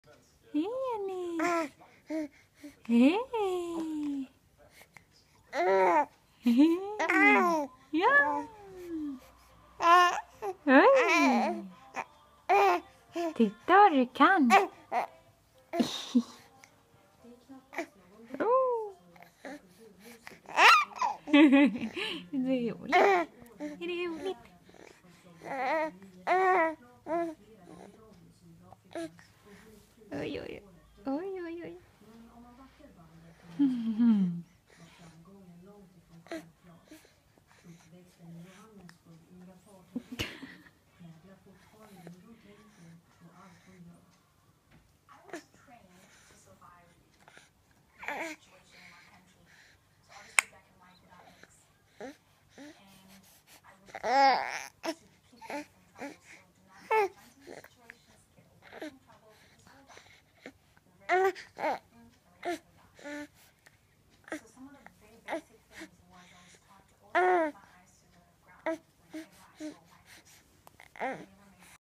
Ennis 10 veckor tittar upp
Ennis är 10 veckor och håller huvudet högt!